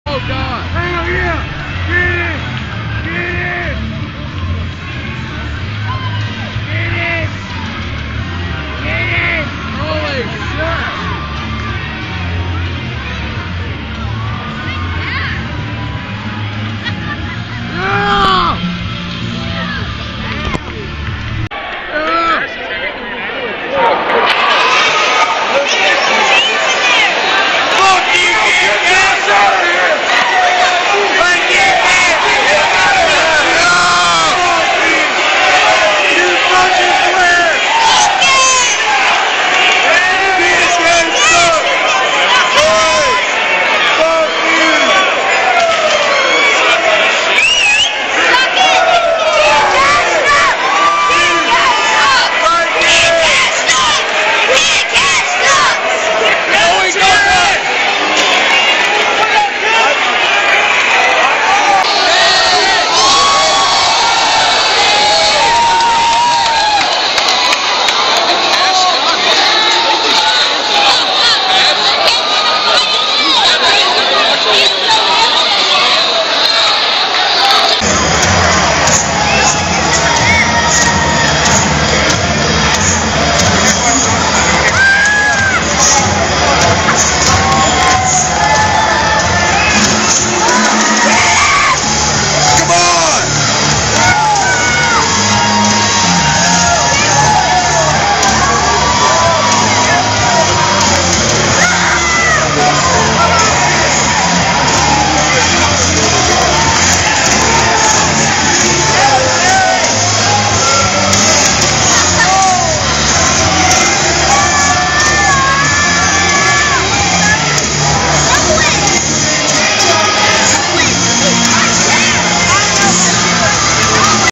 Kid Kash Vs Fans : Nashville TN 8/20/11
Kid Kash having some fights with some of the fans at the Crossfire Wrestling show in Nashville Tennessee on 8/20/11